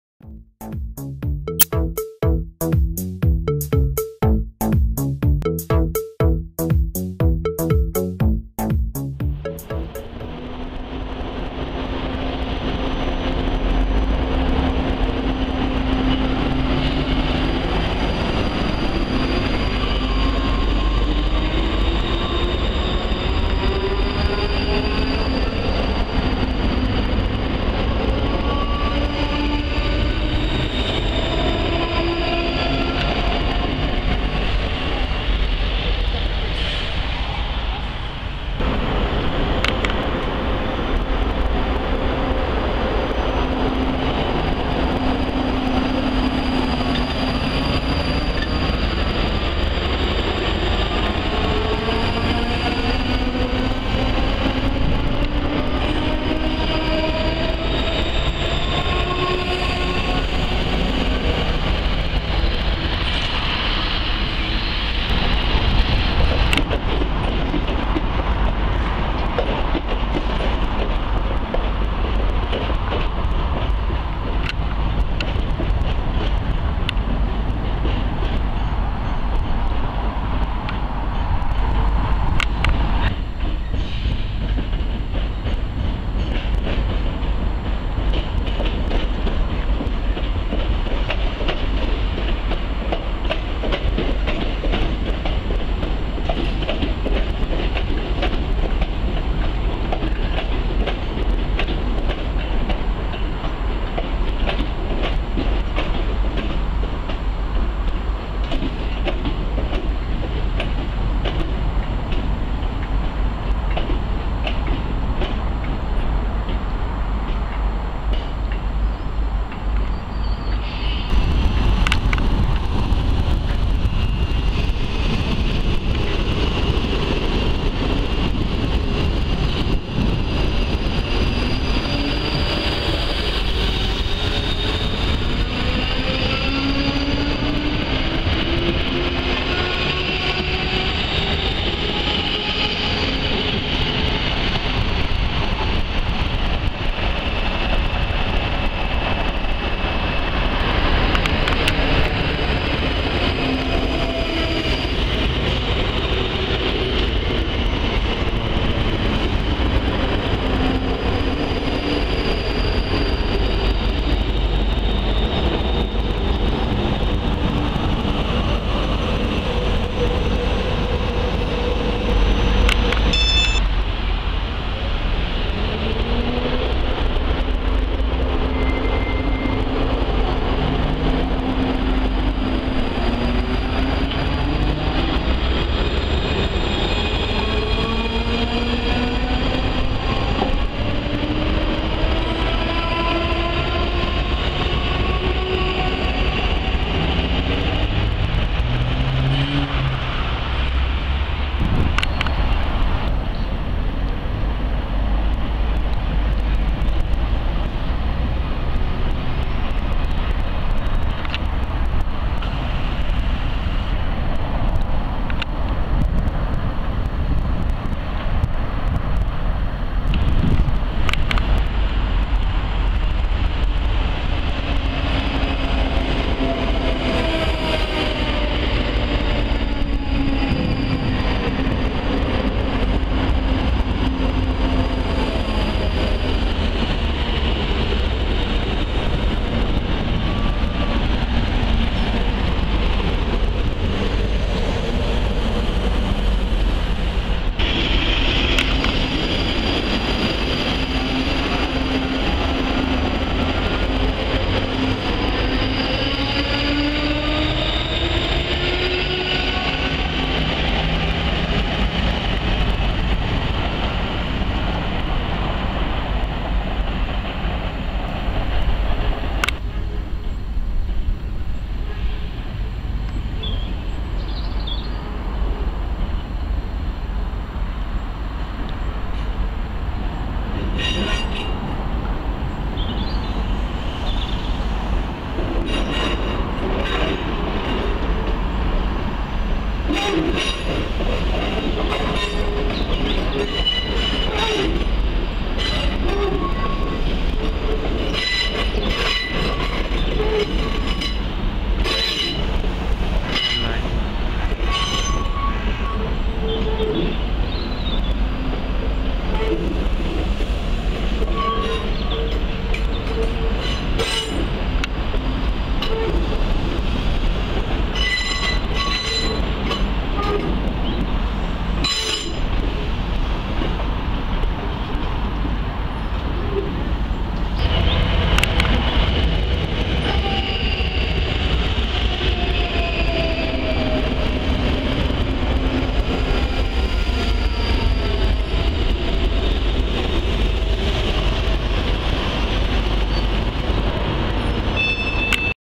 Season 4 Episode 30 - Airdrie
Station number four begins our long journey towards Edinburgh Waverley as we stop off for an hour in the sunshine at Airdrie!